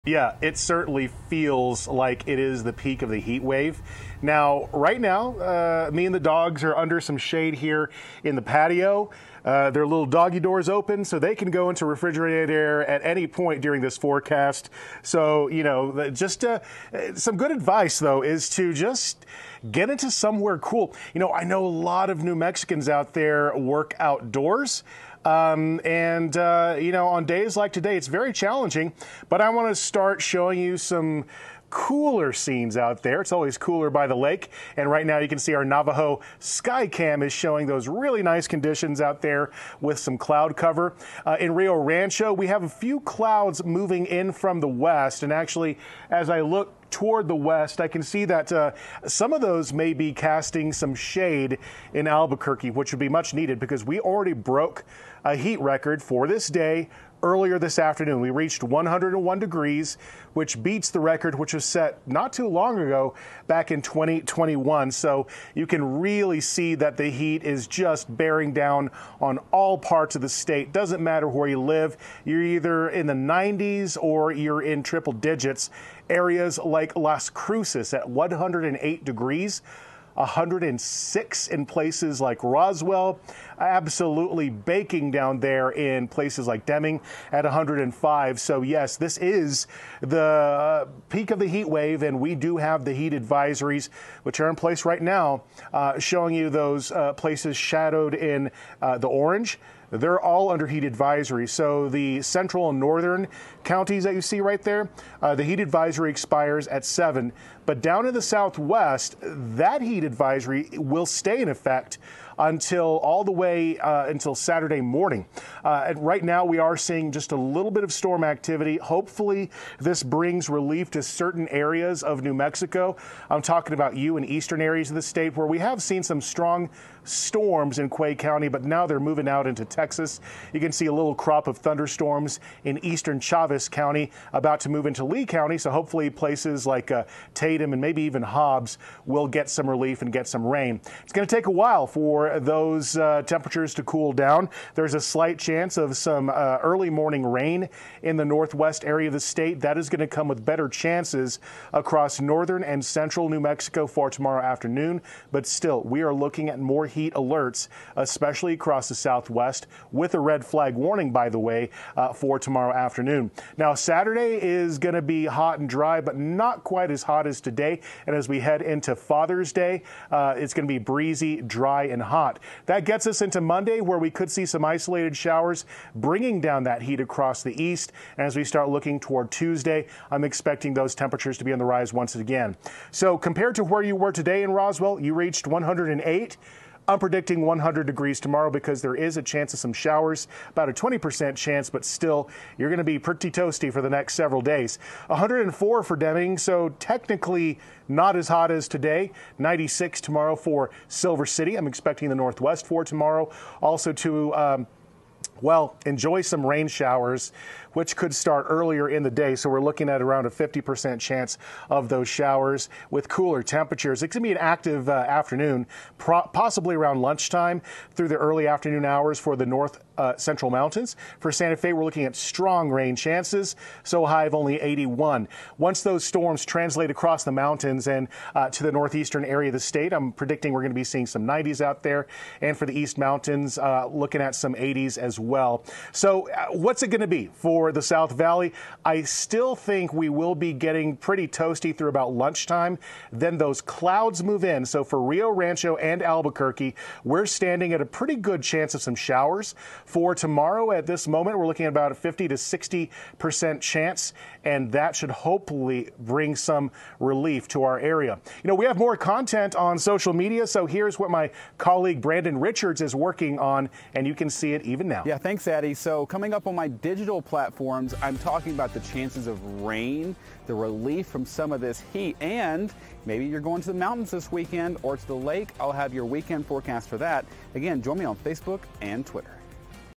Eddie Garcia: Evening forecast for New Mexico | June 13
Here’s the New Mexico weather forecast for Thursday, June 13. For the latest weather alerts, visit KOB.com/weather